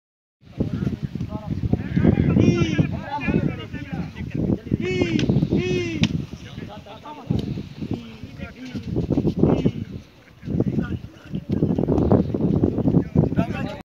/pigeon lover amazing competition video Punjab zila Rahim yar khan/have a good show
good show in Punjab national kabootar bazi amazing video compilation of punjab you are interested